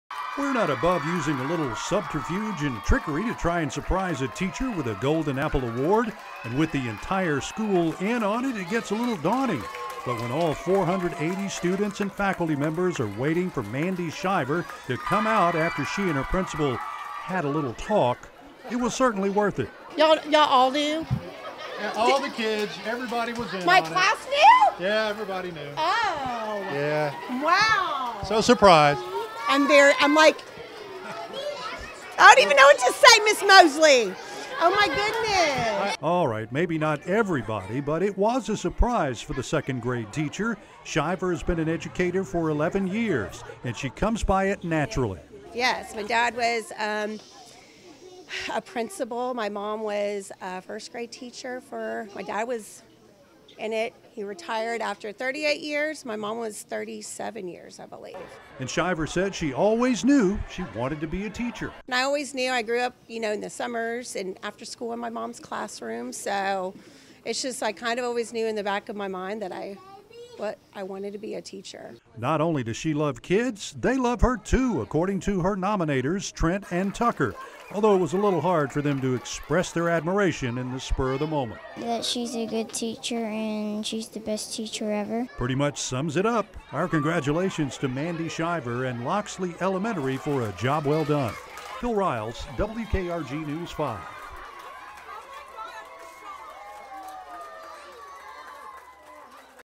Golden Apple Shiver
Mandey Shiver of Loxley Elementary School is this week's Golden Apple Award Winner.